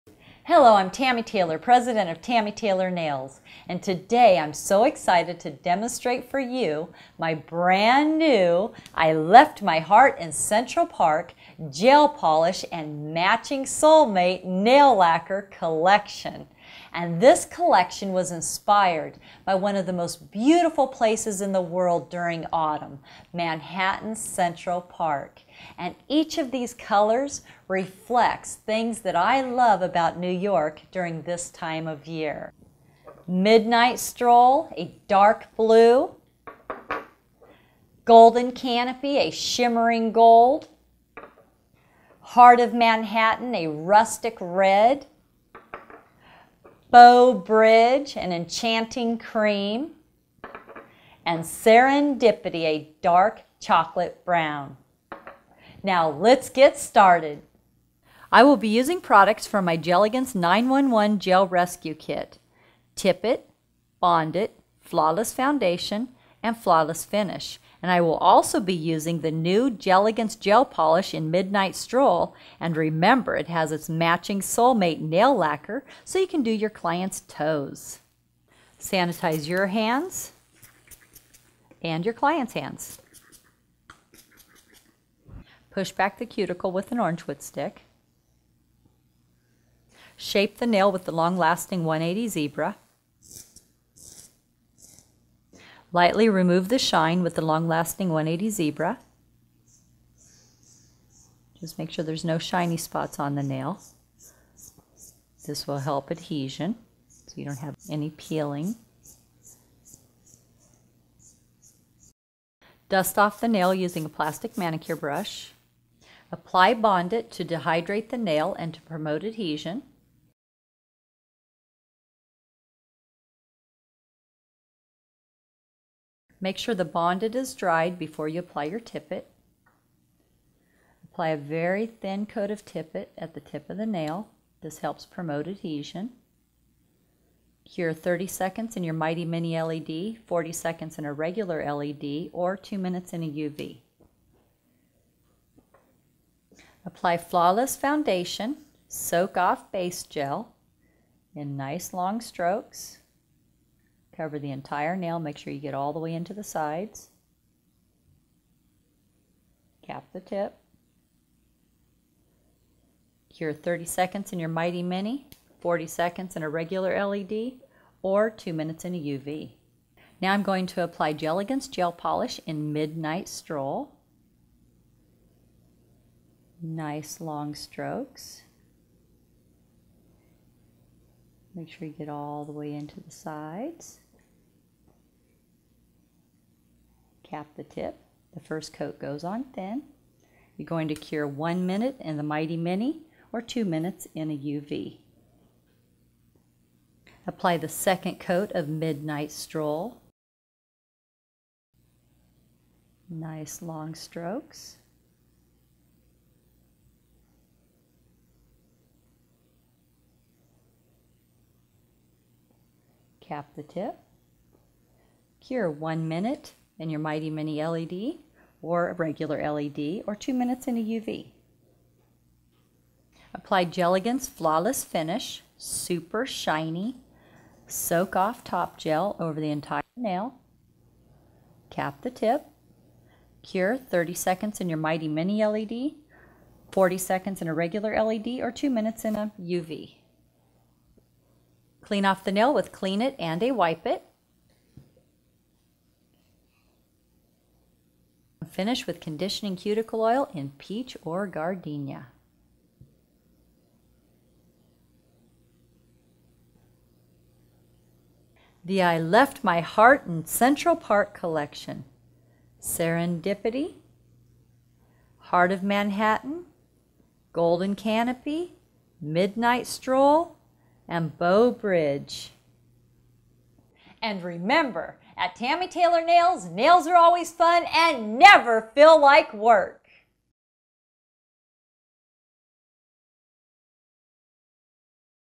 ♡ Tammy Taylor Nail Demo Left My Heart In Central Park Collection
“I am so excited to demonstrate for you the brand new "Left My Heart in Central Park" Gelegance Gel Polish and Matching Soulmate Nail Lacquer Collection.
This new collection was inspired by Autumn in one of the most beautiful and romantic places in the world, New York’s Central Park, a canvas of beautiful colors, crisp air, layers, delicious treats and romantic walks.
Which is why every color in my "Left My Heart in Central Park" Collection reflects on things I love about Manhattan during Autumn:  Bow Bridge- an enchanting crème, Midnight Stroll- a deep blue, Golden Canopy- a shimmering gold, Heart of Manhattan- a rustic red, and Serendipity- the perfect chocolate brown. Together these colors are the essence of Autumn, which color will capture your heart this season?”

Have Fun,
Tammy Taylor -